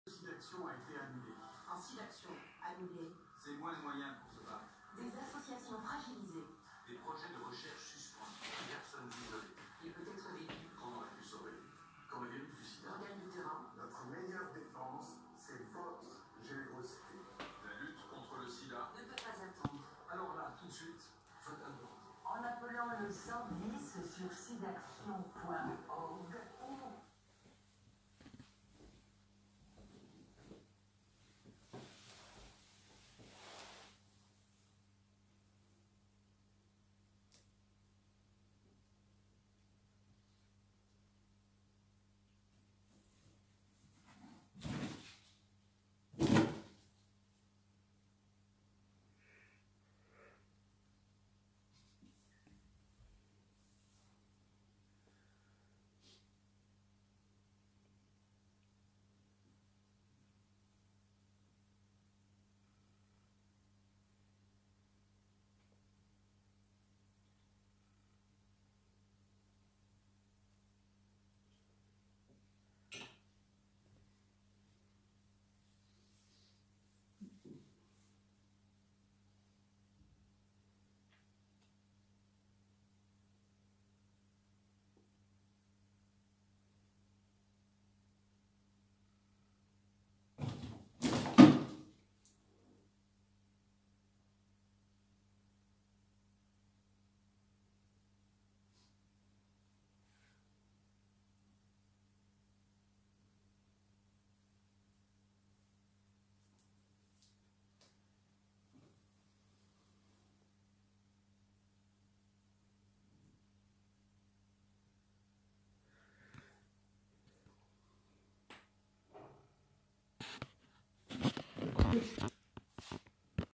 Deux chats. Un carton. Deux issues.
Cosette est cernée dans sa cachette cartonnée. L'agent Jispy surveille toutes les issues. Aucune échappatoire possible.